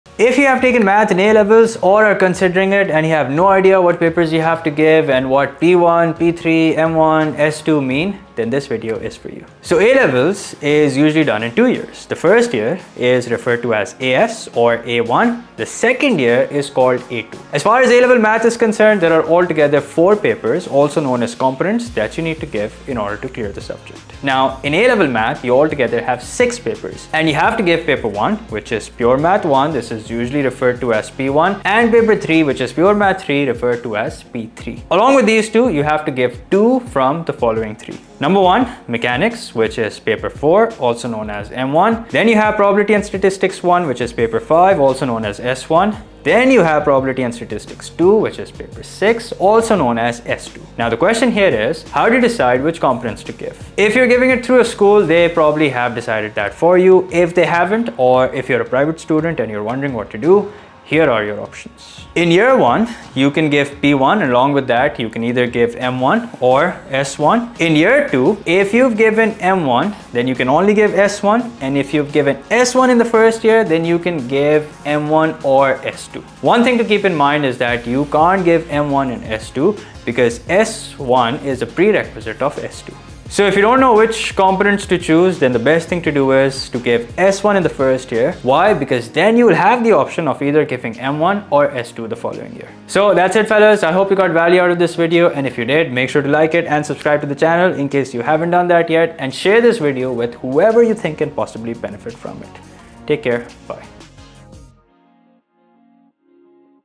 A-Level Math Explained: Paper Choices Explained 🌟
Are you feeling lost when it comes to A-Level Math papers? Don’t worry, we’ve got you covered! 💡 In this video, we’ll walk you through the key components of A-Level Mathematics, breaking down the different papers (P1, P3, M1, S1, and S2) and guiding you on how to make the best choices based on your needs—whether you're a school or private candidate.

By the end of the video, you’ll have a clear understanding of which papers to focus on and how to approach them. This is your go-to guide for A-Level Math success! 🌟

Have any questions? Drop them in the comments! Let’s tackle it together.

To get in touch regarding A Level Math (9709), IGCSE Math (0580) O level Math (4024) and Add Math (4037):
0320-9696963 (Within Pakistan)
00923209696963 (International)